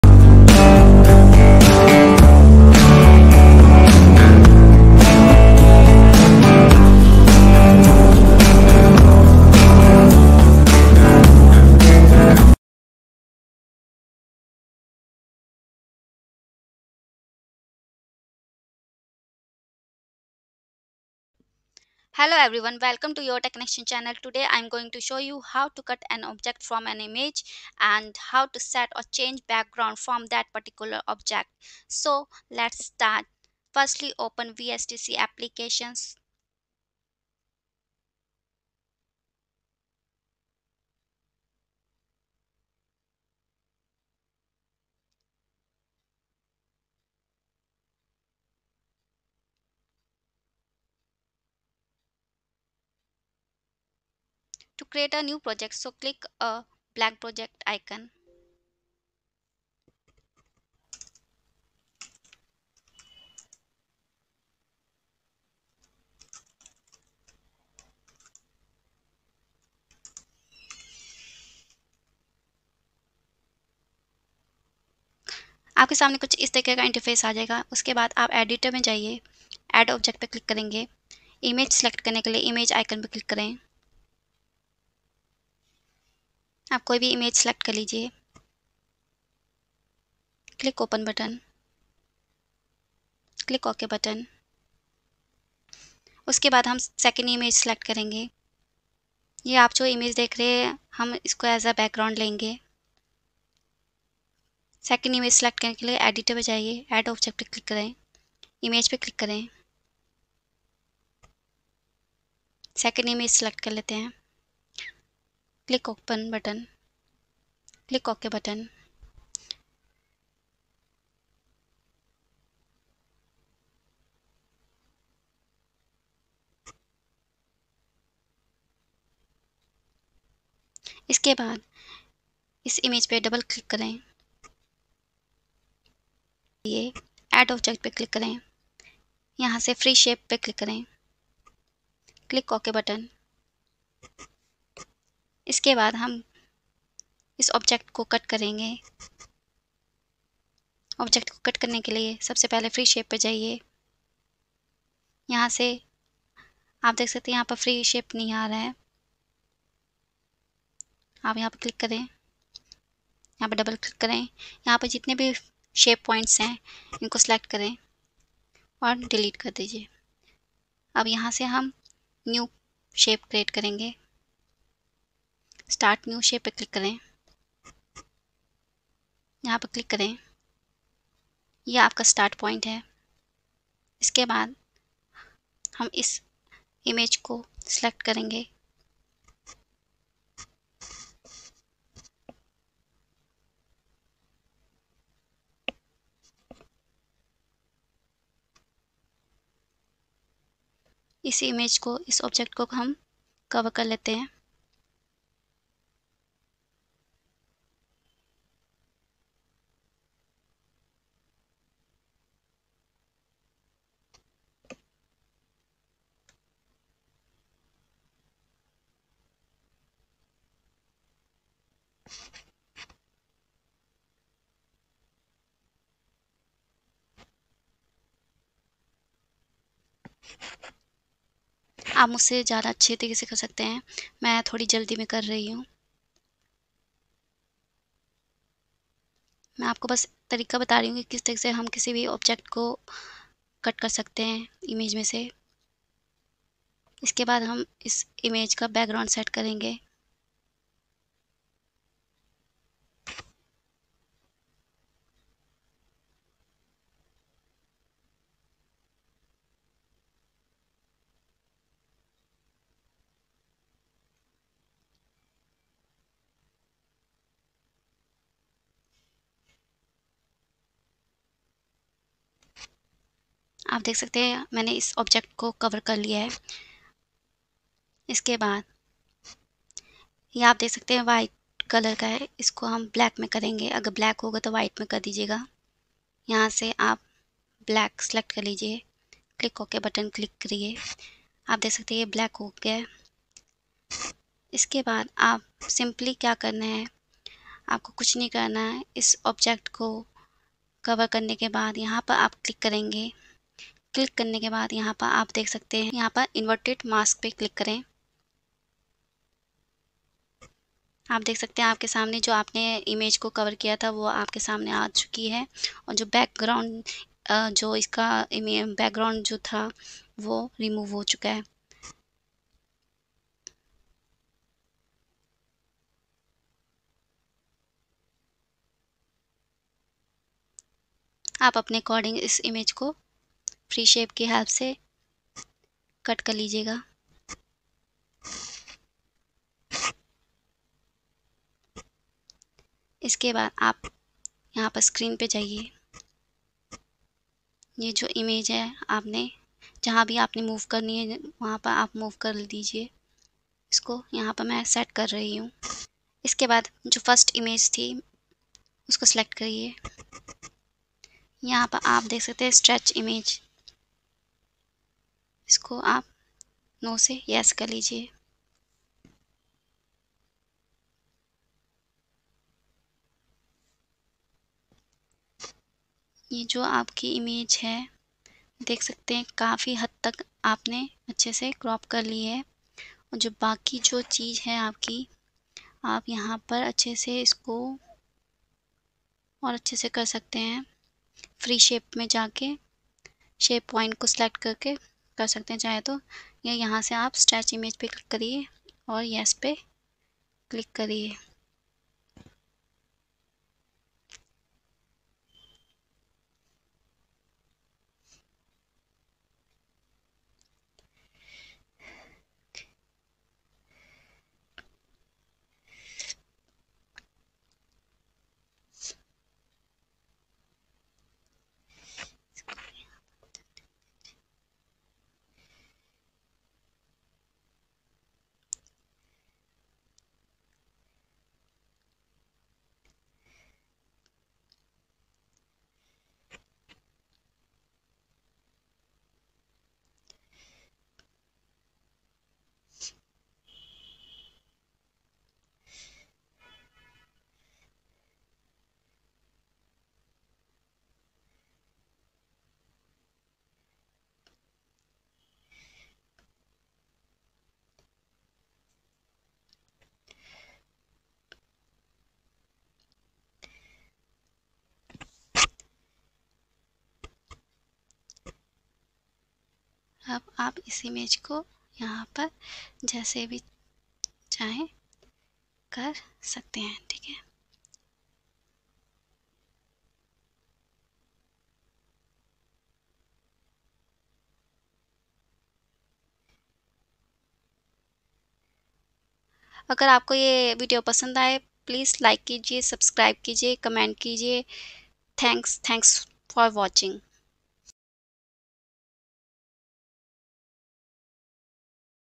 How to cut an object from a image | change the background | VSDC [ Free ]
00:00 How to cut an object from a image and change the background | VSDC [ Free ]

Panther Intro Templates :-